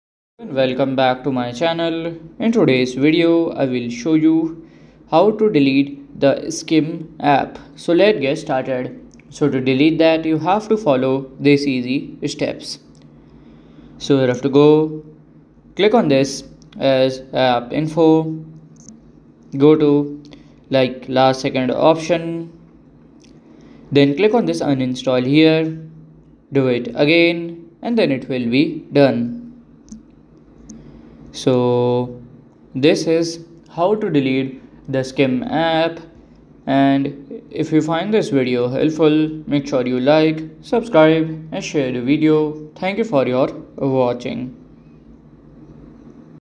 ✅ How To Uninstall/Delete/Remove theSkimm App (Full Guide)
In this video, we provide a comprehensive guide on how to uninstall, delete, or remove the theSkimm app from your device. Follow along to successfully remove the app.

Key Learning Points:
 • Step-by-step uninstallation process
 • Troubleshooting tips
 • Complete app removal